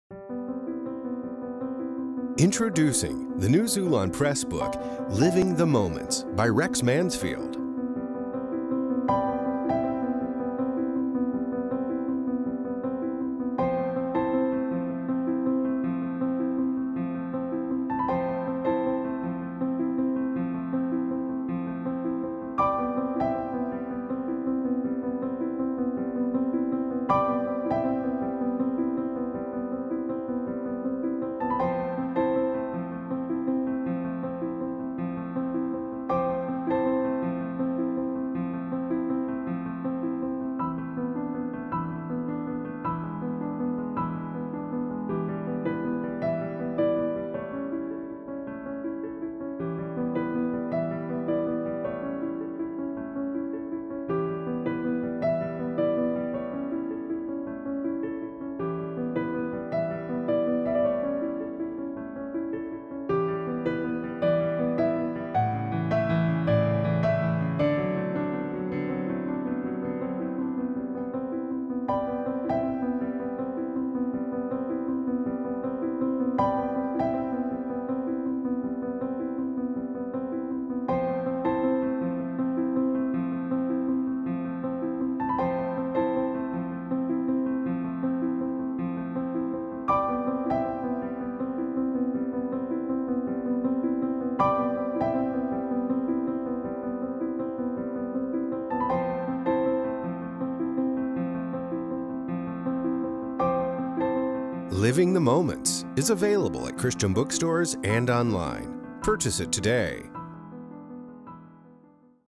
Xulon Press book LIVING THE MOMENTS - My Journey with Elvis, Elisabeth and Jesus | Rex Mansfield
Xulon Press introduces LIVING THE MOMENTS - My Journey with Elvis, Elisabeth and Jesus by author Rex Mansfield. The Xulon Press bookstore provides an easy way to purchase this book using the link below, or you can find it at your local Christian bookstore.

Category:
Entertainment